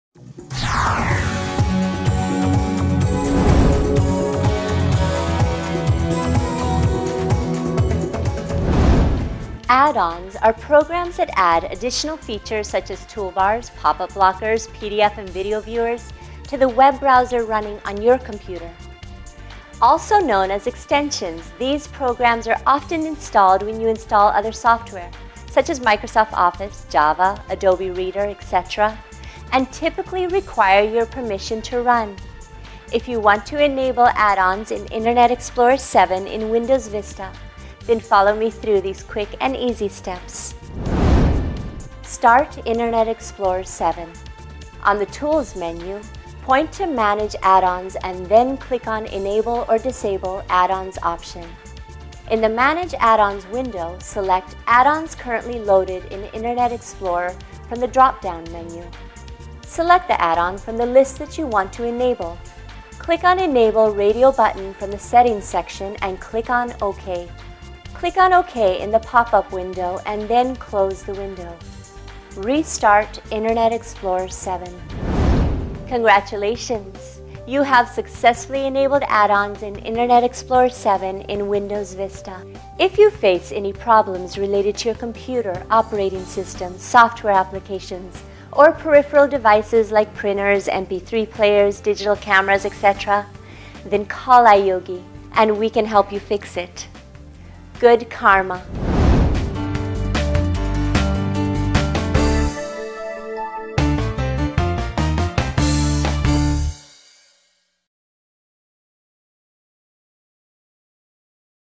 How to enable add-ons in Internet Explorer® 7 in Windows® Vista
Learn how to enable add-ons in Internet Explorer® 7 in Windows® Vista-based computer system.